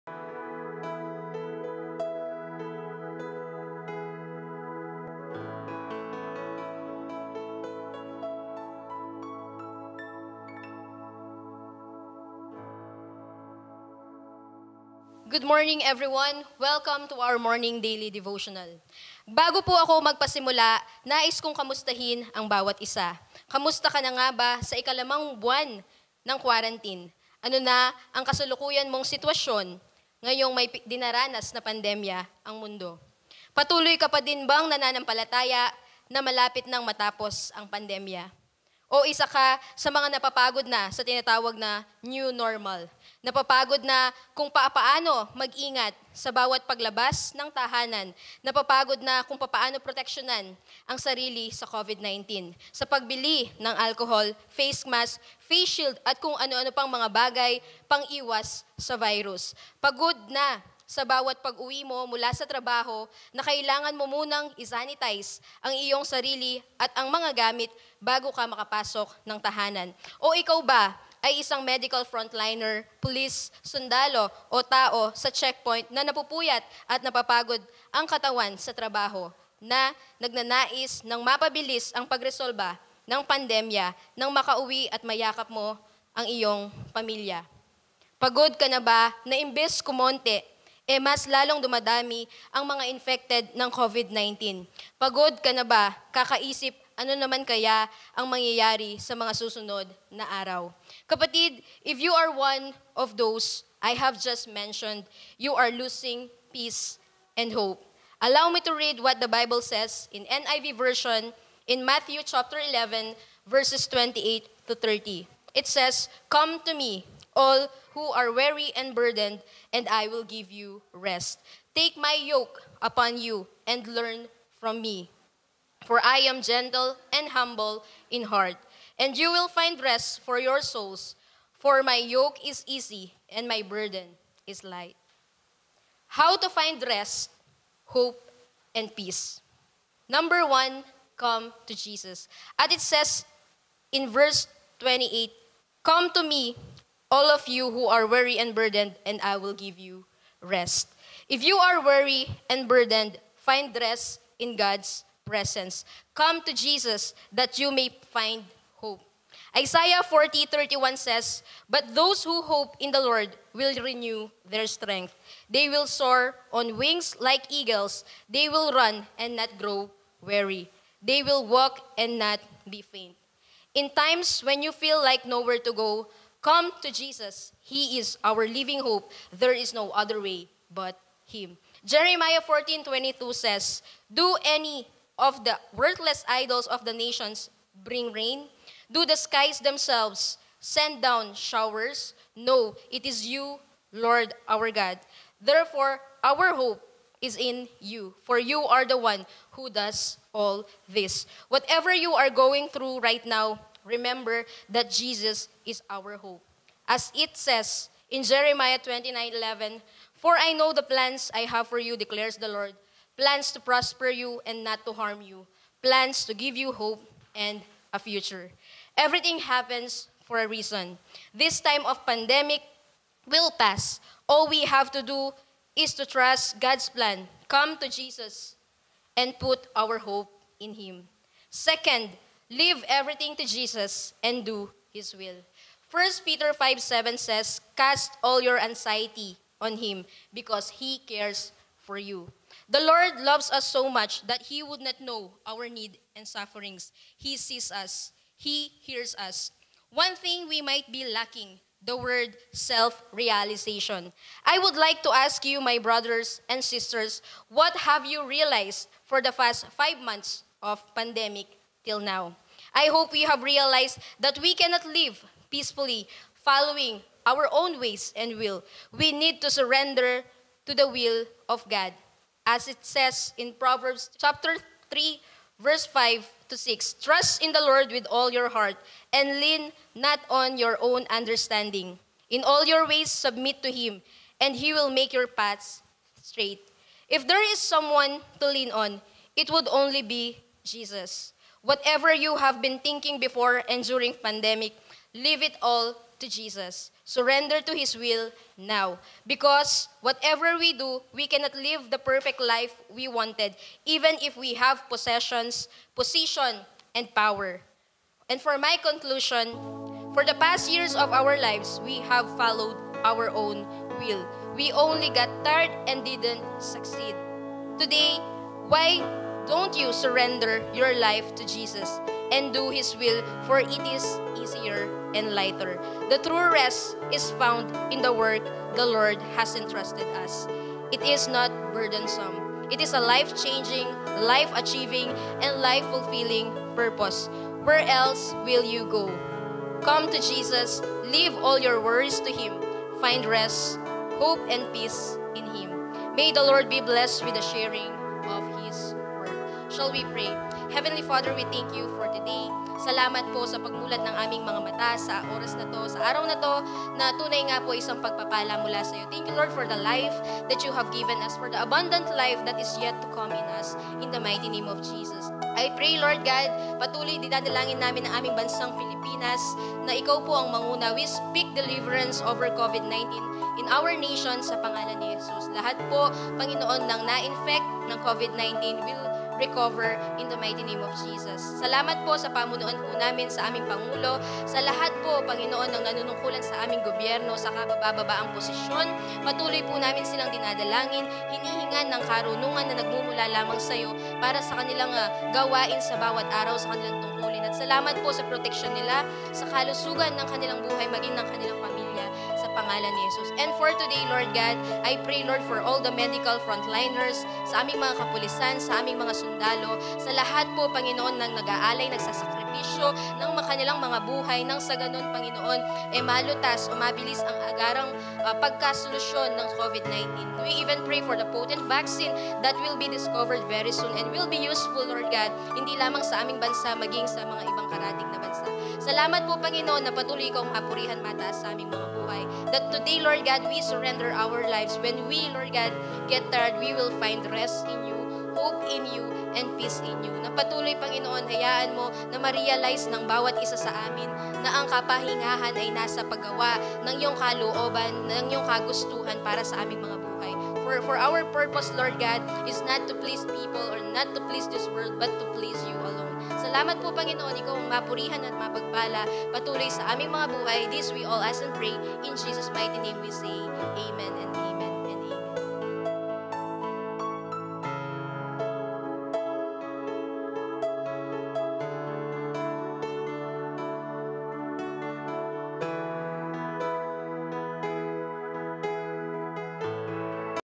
FIND REST IN HIM | DAILY DEVOTIONAL | 13AUG20
With the tough days that we are striving to win daily, we all need rest, and we can only truly appreciate it when God is the source of it. As we enjoy the work of the Lord and His Word in our lives, may we find the serenity to push through the next level of our tasks, May we hear in silence His beautiful call that we can make it!